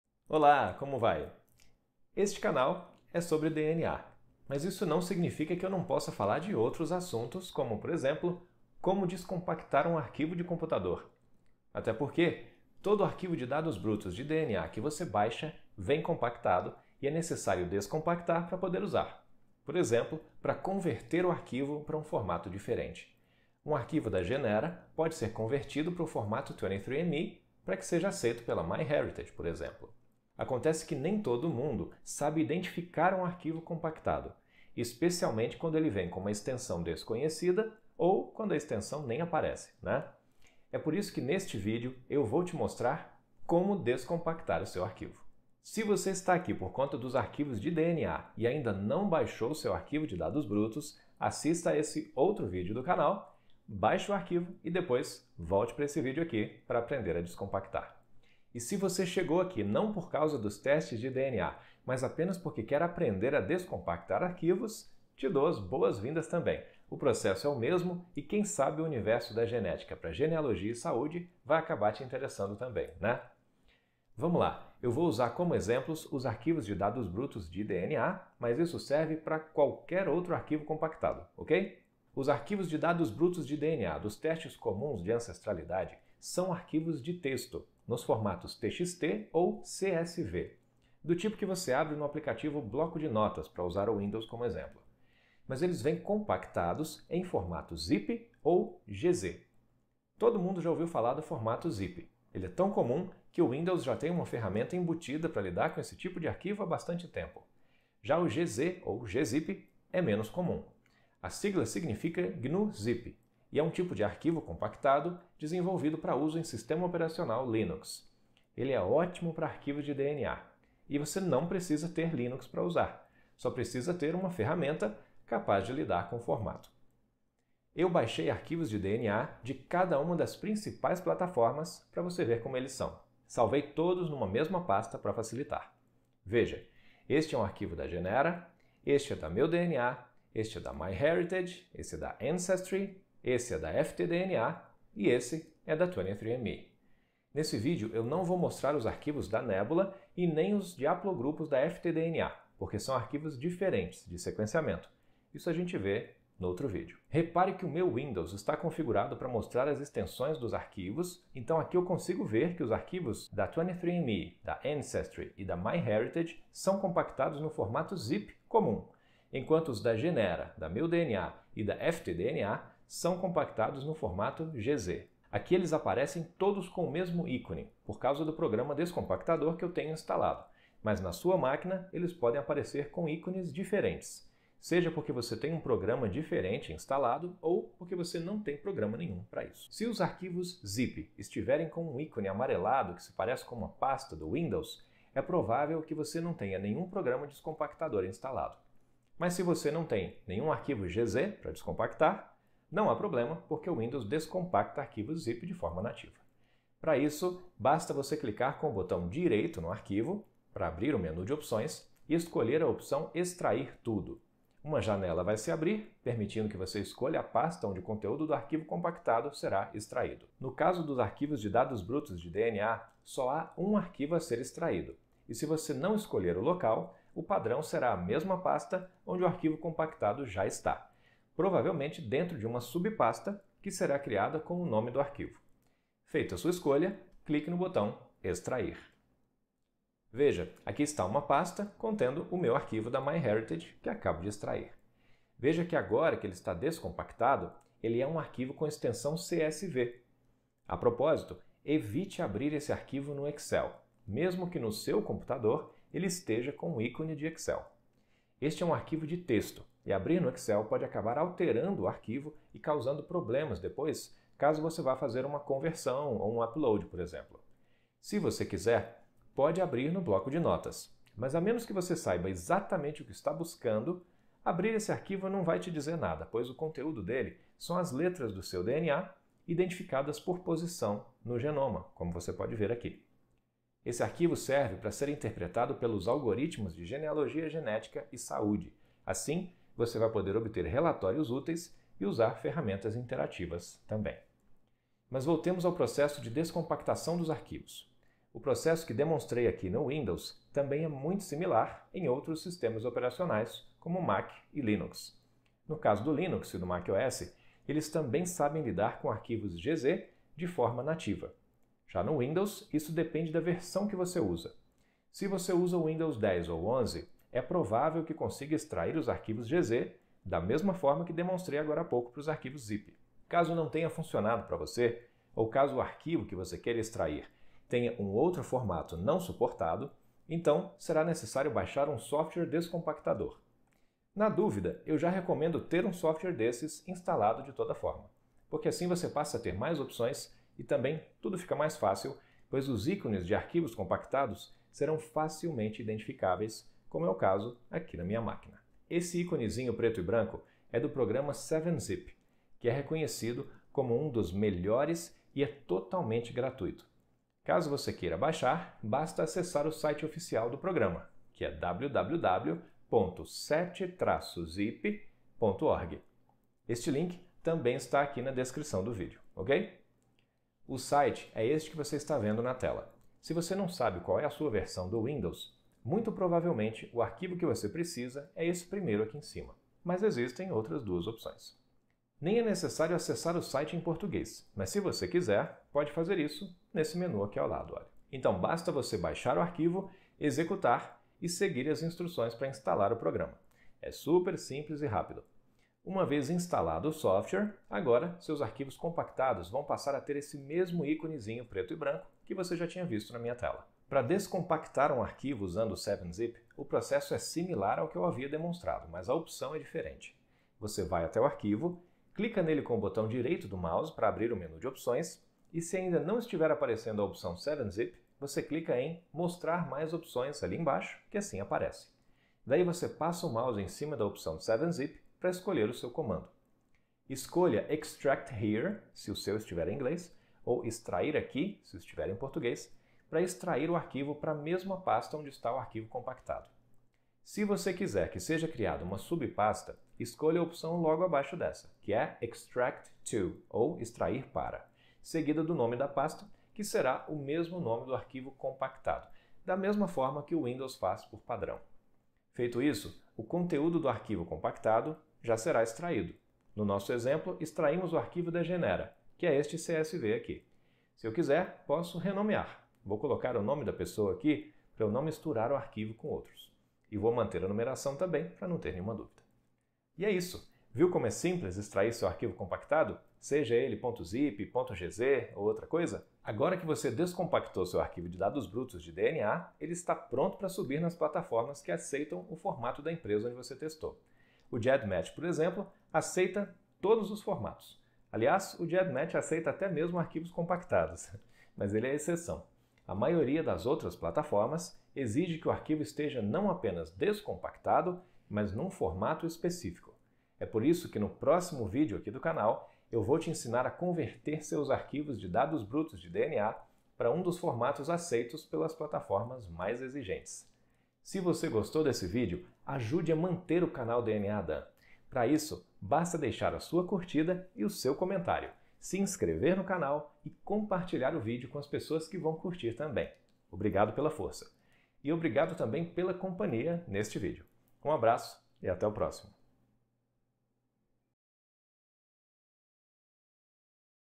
Como descompactar arquivos (de dados brutos do DNA ou qualquer outro)
Nem sempre é fácil perceber quando um arquivo está em formato compactado e nem todo mundo reconhece um arquivo com extensão .GZ, por exemplo (especialmente quando essa extensão nem aparece). Neste vídeo, Dan vai te ensinar a descompactar seu arquivo, seja ele de dados brutos de DNA ou de qualquer outra coisa. É mais fácil e rápido do que você imagina!

A propósito, TODOS os arquivos de dados brutos de DNA que você baixa dos sites das empresas vêm compactados, seja em formato .ZIP ou .GZ. É de dentro desses arquivos compactados que você extrai o arquivo de dados brutos propriamente dito, que tem formato .TXT ou .CSV. Assista para aprender como fazer a descompactação e deixar seu arquivo pronto para as próximas etapas, como, por exemplo, a conversão de formato. 😉🧬

0:00 - Introdução
1:13 - Tipos de arquivos de DNA
3:10 - Descompactando no Windows
4:10 - Abrindo seu arquivo de dados brutos de DNA
4:55 - Outras plataformas e formatos
5:24 - Baixando o 7-Zip
6:44 - Descompactando arquivos com o 7-Zip
7:50 - Conclusão